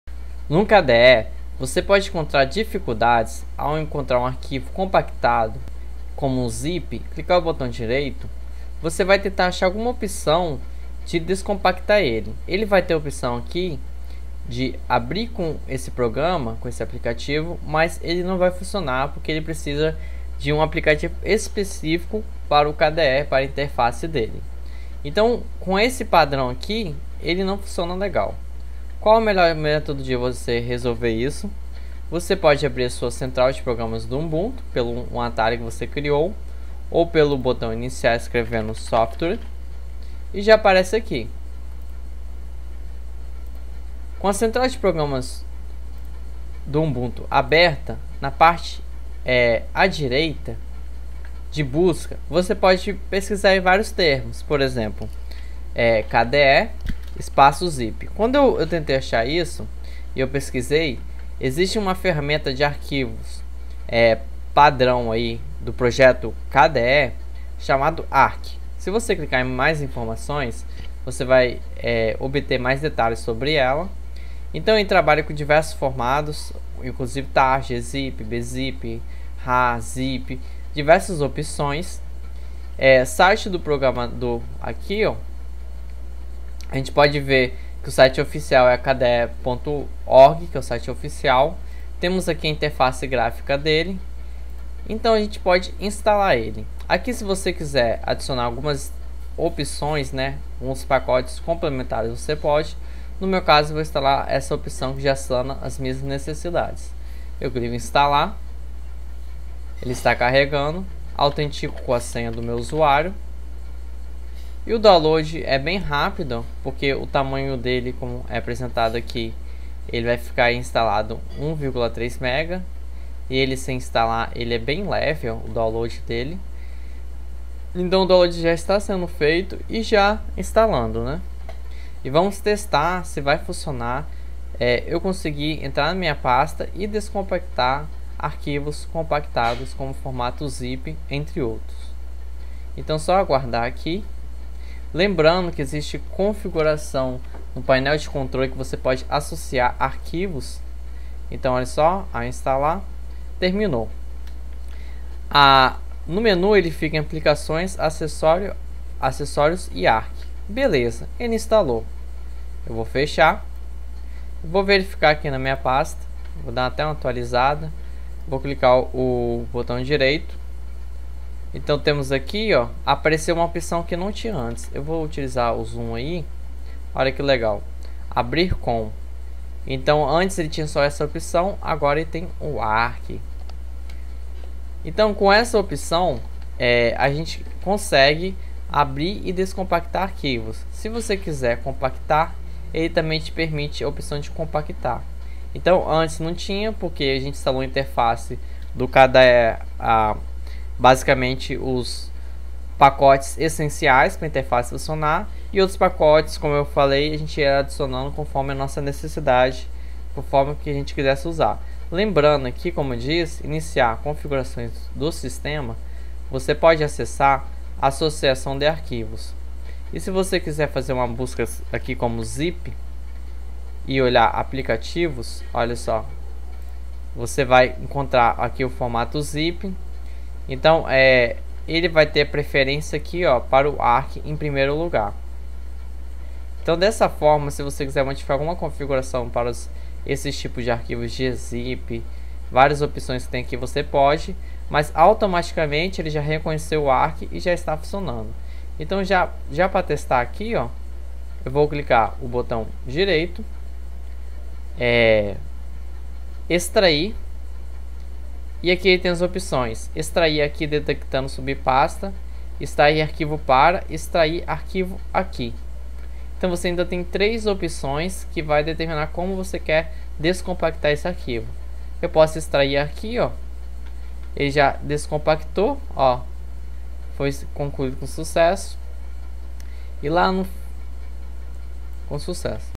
Curso de Linux Ubuntu - 58 - Ark descompactador de arquivos no KDE
Vídeo aula de Linux Ubuntu - 58 - aprenda como compactar e descompactar arquivos na interface KDE.